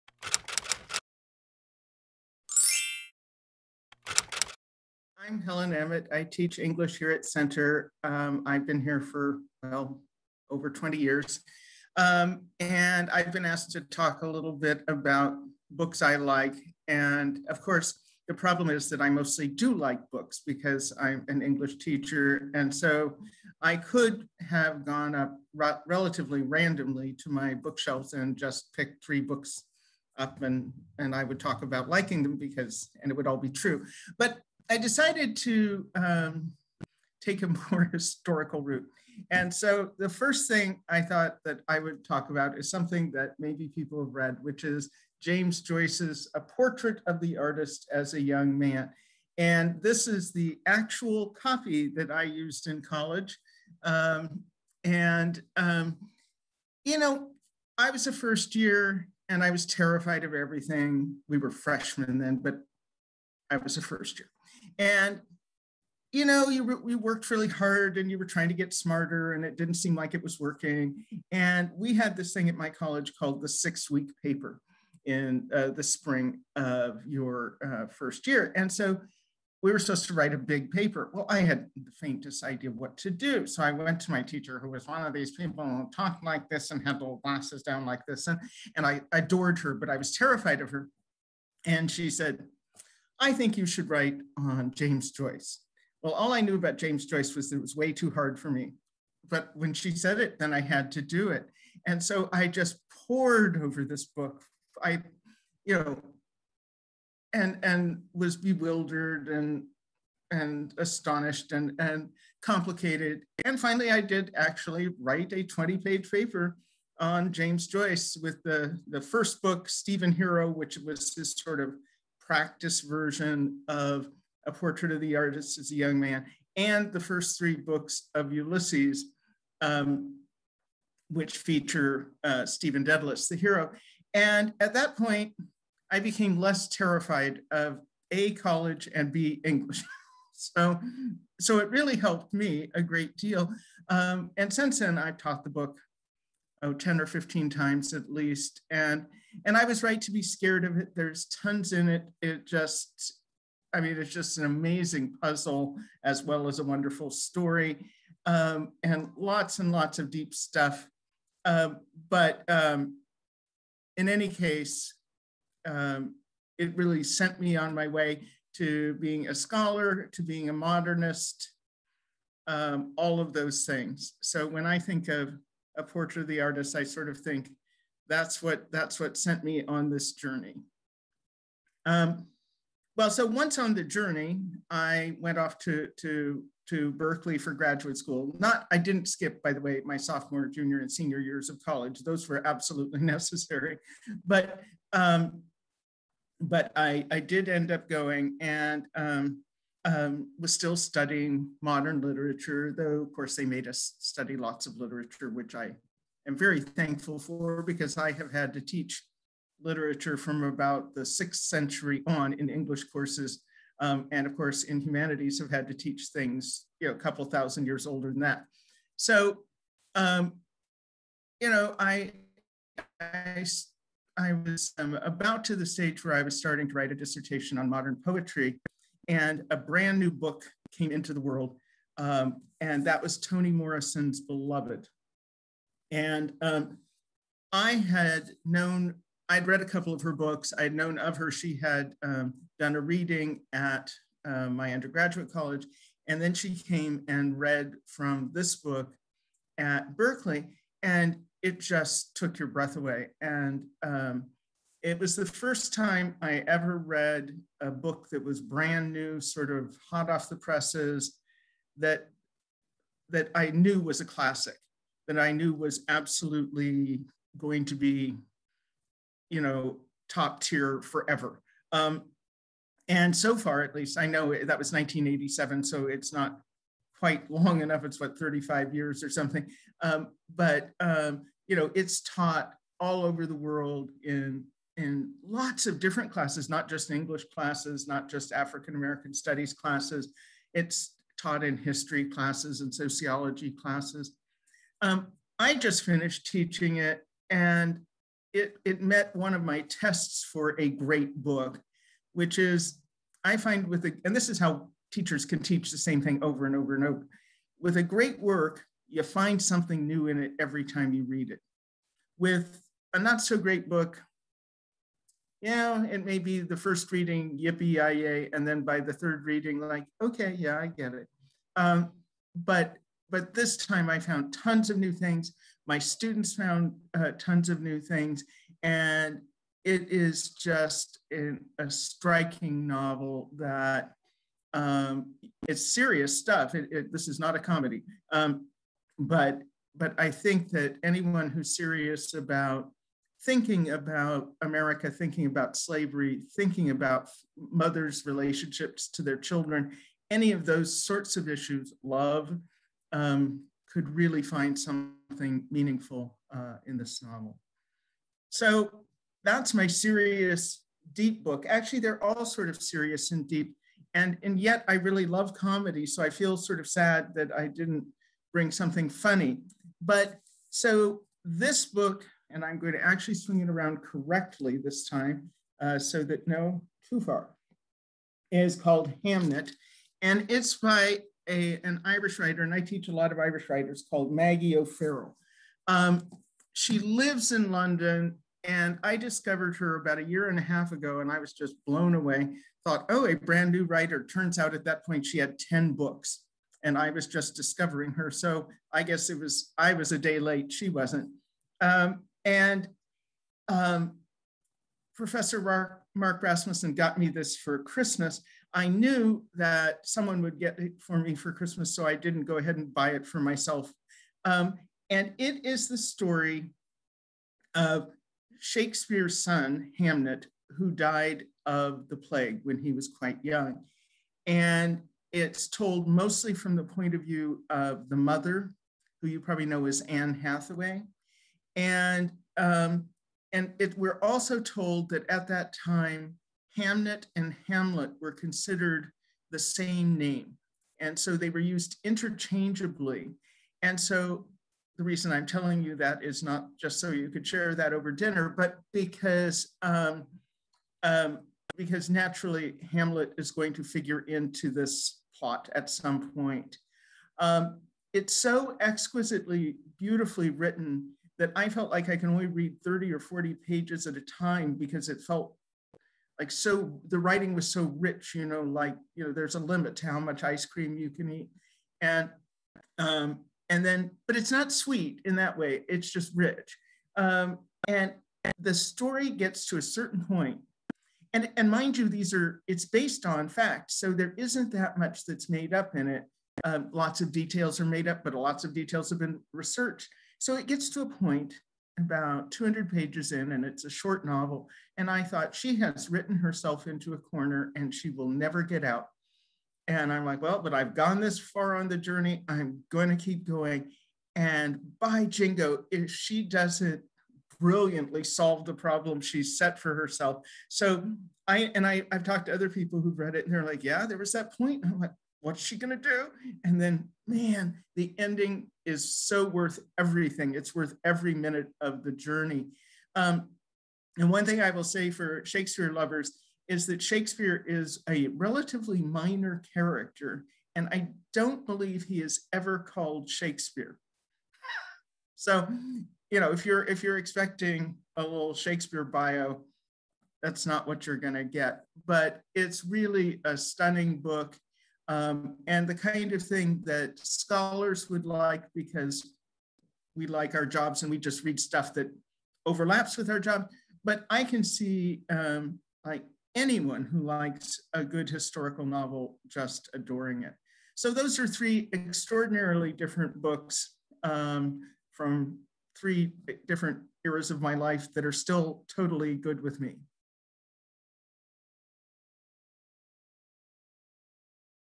Centre Reads with Dr. Helen Emmitt
Dr. Helen Emmitt shares three books from her bookshelf.

-A Portrait of the Artist as a Young Man by James Joyce
-Beloved by Toni Morrison
-Hamnet by Maggie O'Farrell